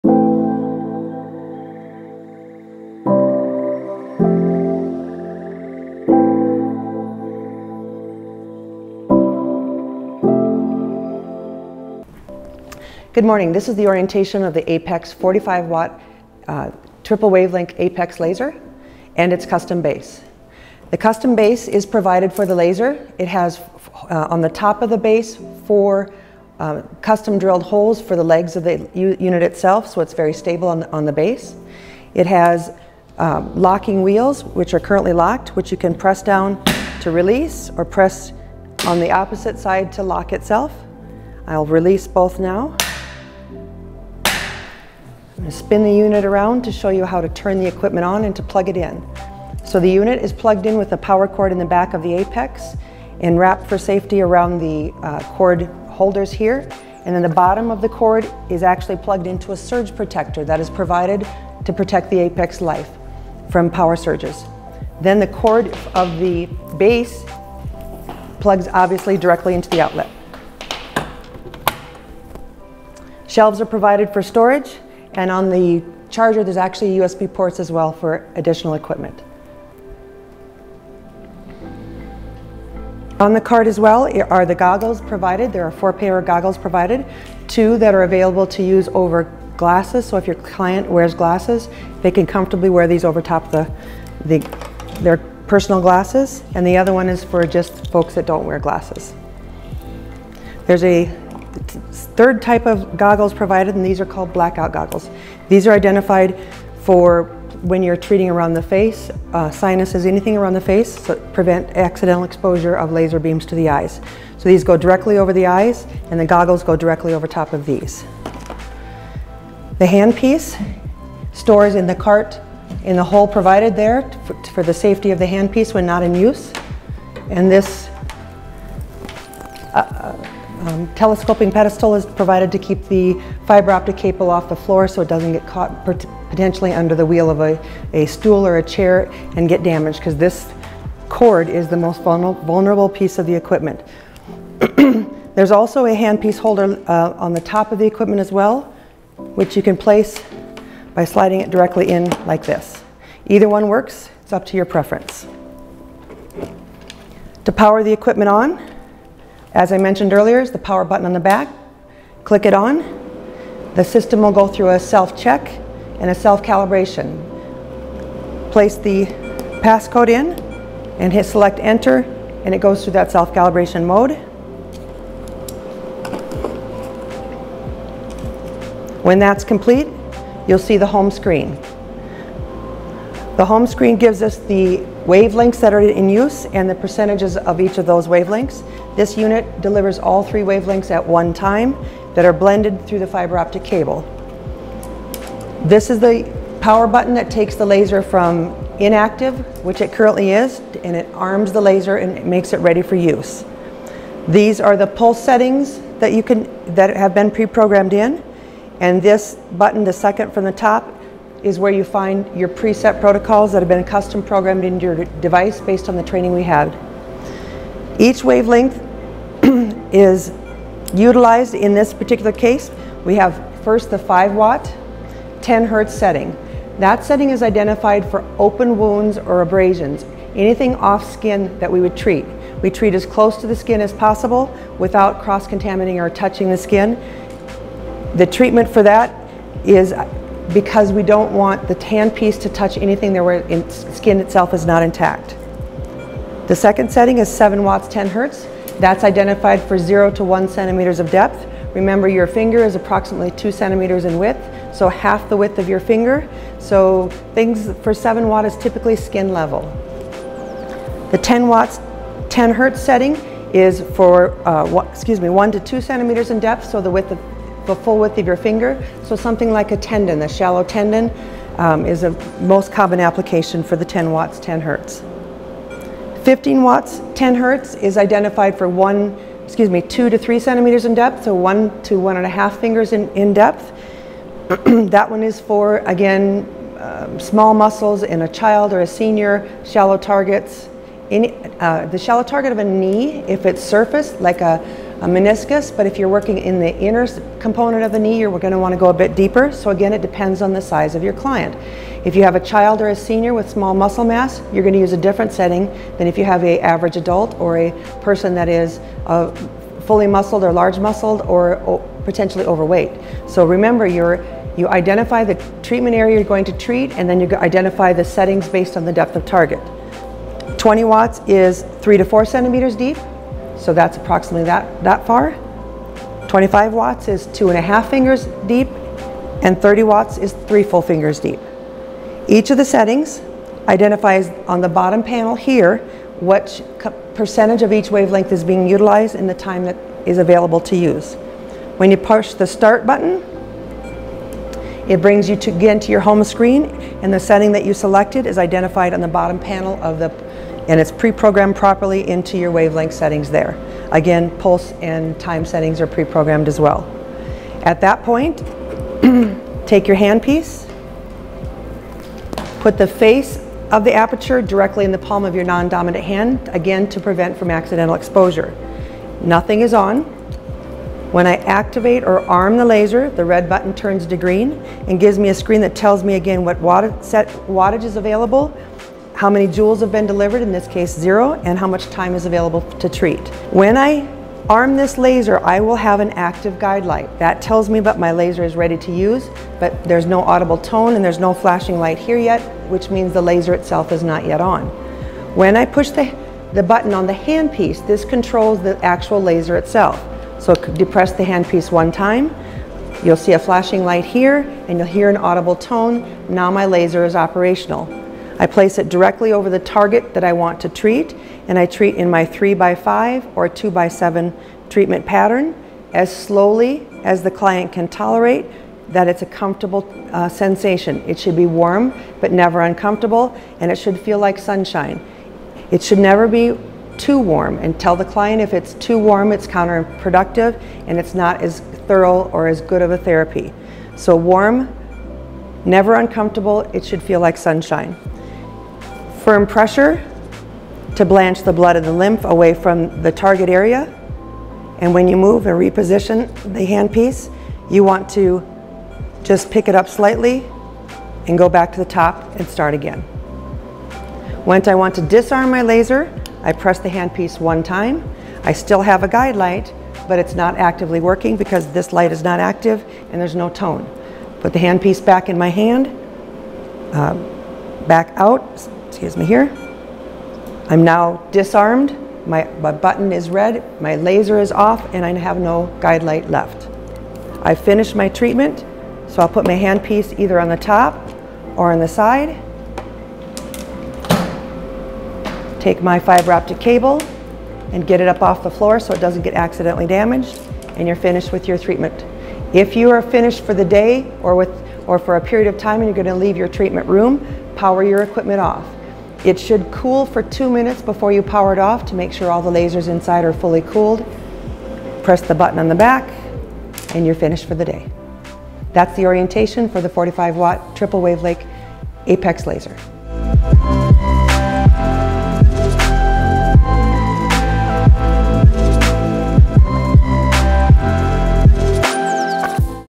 Aspen Laser Orientation: Apex Laser
With over 35 years of experience in Photomedicine, Aspen Laser has developed the perfect Class IV Laser Therapy System for your practice. Combining three proven therapeutic wavelengths (810nm, 980nm, 1064nm), with an easy to use interface and ergonomic therapy handpiece, the Aspen Laser delivers effective results in minutes.